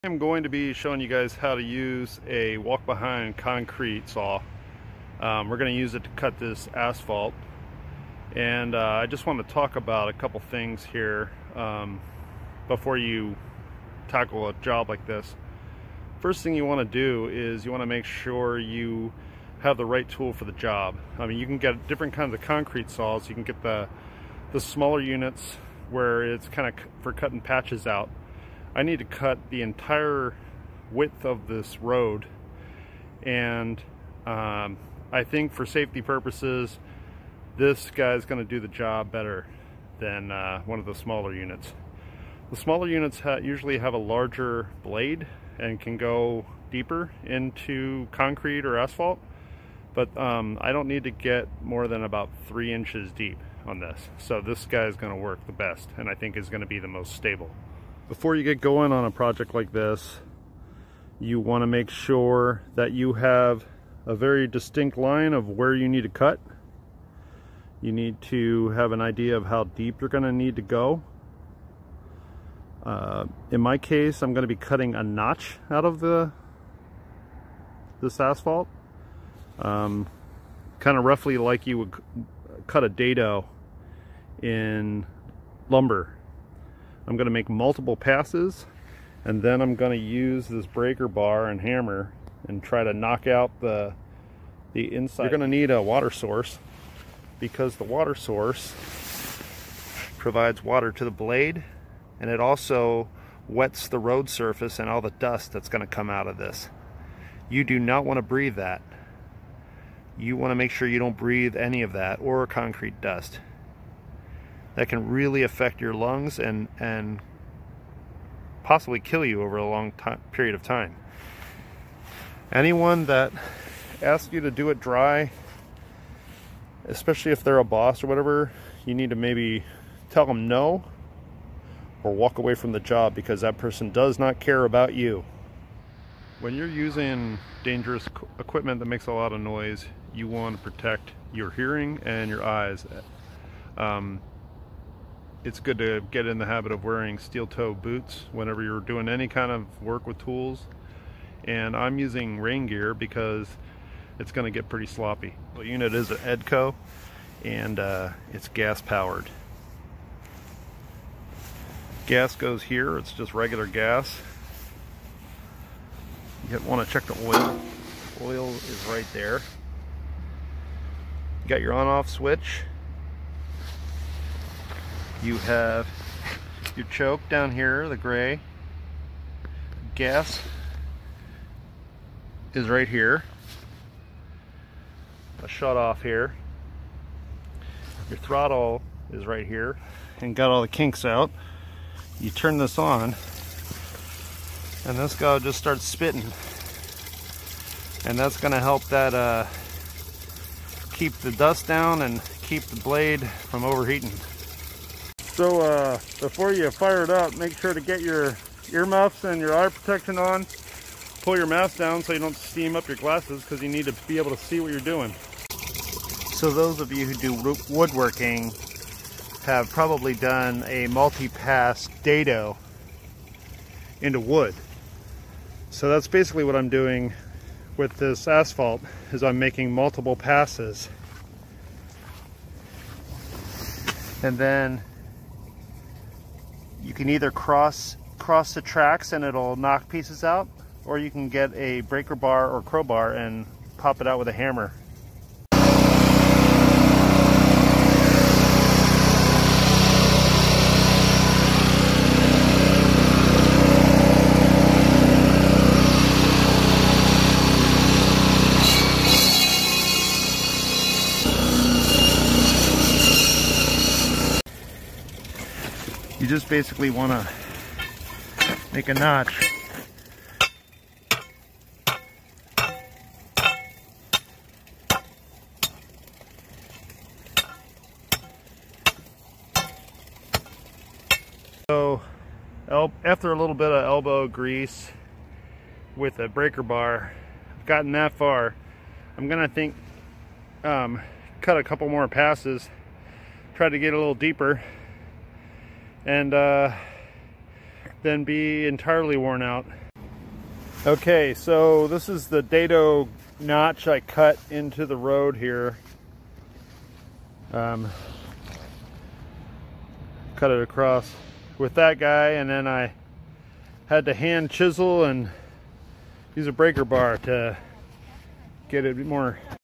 How to Use a Walk Behind Asphalt Cutter and Concrete Saw
In this video, I show how I cut a 30 foot long groove into an asphalt road. Instead of the more commonly known Stihl and Milwaukee handheld concrete saws, I opted to use a larger, walk behind EDCO concrete wet saw, and did my work by cutting the groove in multiple passes, like you would a dado in wood with a skilsaw.

I am trying to install an ethernet fiber optic cable under the road for a facilities project and didn't want to direct bore it.

An Asphalt cutter is a really big circular saw that can cut hard surfaces, like concrete slab, asphalt, and other kinds of sidewalk, pavement and road materials. It utilizes a water hookup to keep the blade and concrete material wet so friction doesn't burn up the blade and the dust is kept down and out of your lungs.

Using one of these saws without water is dangerous as the dust produced can get into your lungs and eyes and damage your mucous membranes and breathing.  Use caution when working around one of these machine operated while dry.